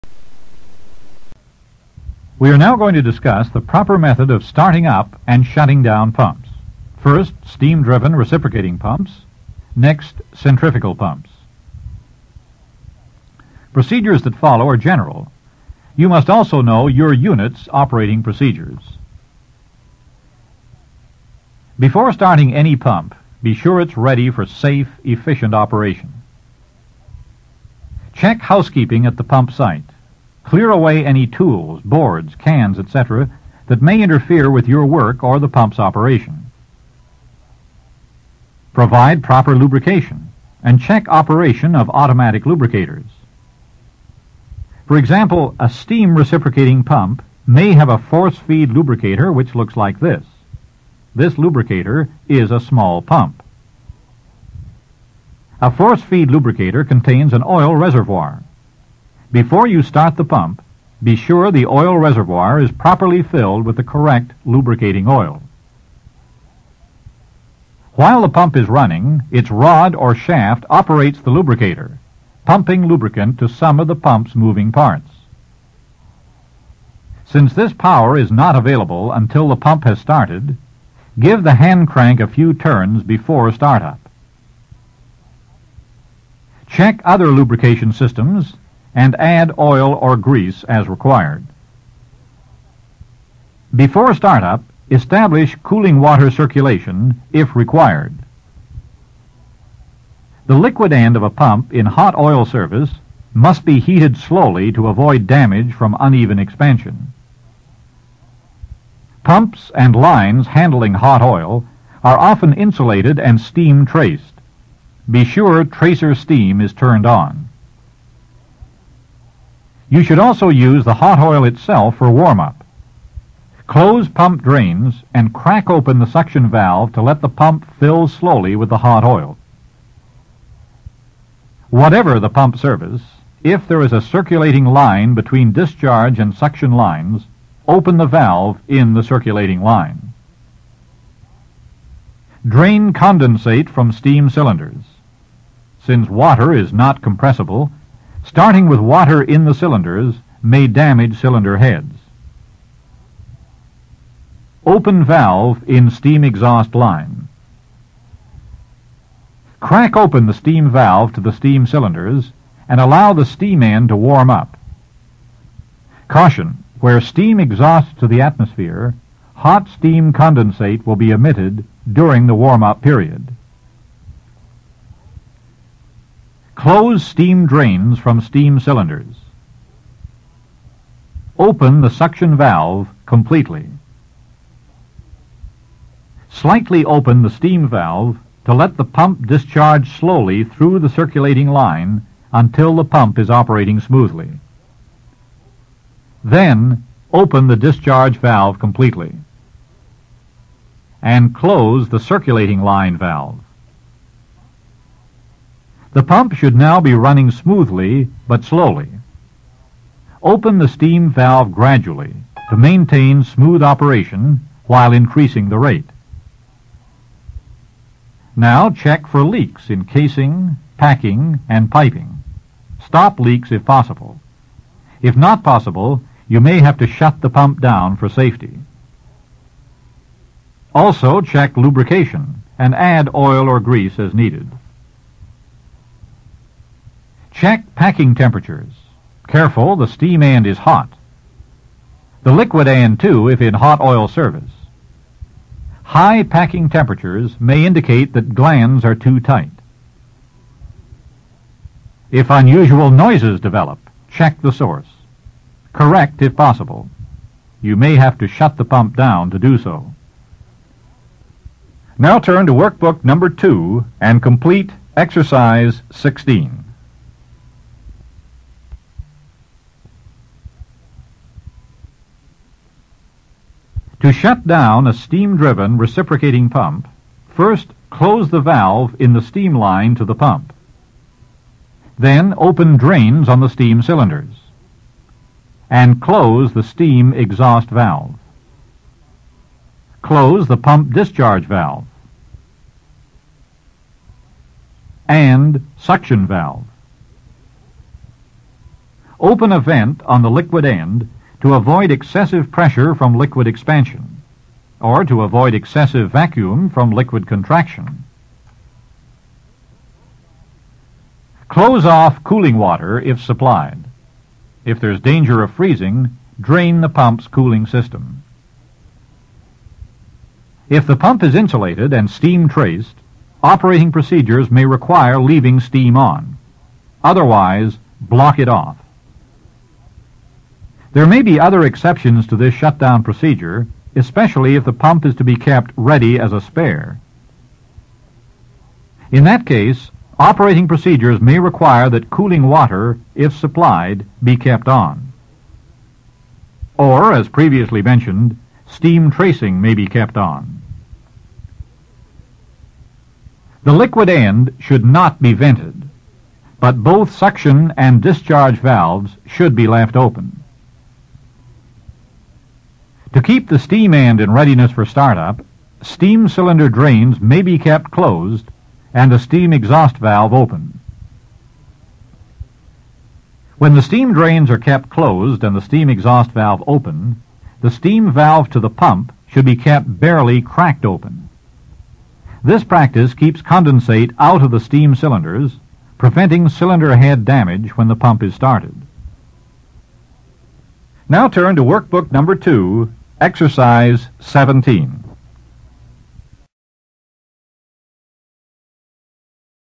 Pump Part 7 | Centrifugal & Positive Displacement pump Concept | Pump Works | Pump Operation
Pump Part 7
Centrifugal & Positive Displacement Pump Module 1.29 Slide 1 to 10.
Segment 25
How Centrifugal pumps operate and Works.
How Positive Displacement Pump operates and Works.
Pump Startup and Shutdown